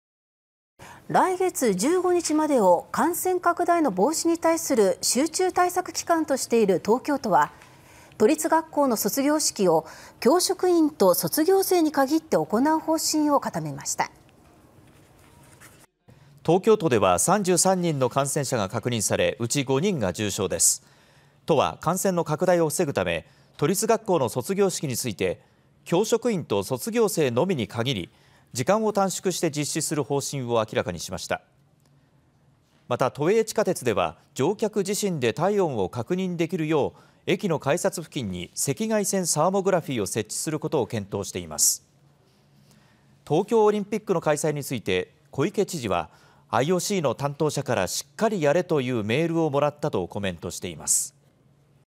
都立学校の卒業式 保護者なし時間短縮で実施の方針(20/02/26)
来月15日までを感染拡大の防止に対する集中対策期間としている東京都は、都立学校の卒業式を教職員と卒業生に限って行う方針を固めました。

　東京都では33人の感染者が確認され、うち5人が重症です。都は感染の拡大を防ぐため、都立学校の卒業式について教職員と卒業生のみに限って時間を短縮して実施する方針を明らかにしました。また、都営地下鉄では乗客自身で体温を確認できるよう駅の改札付近に赤外線サーモグラフィーを設置することを検討しています。
　東京オリンピックの開催について小池知事は「IOC（国際オリンピック委員会）の担当者からしっかりやれというメールをもらった」とコメントしています。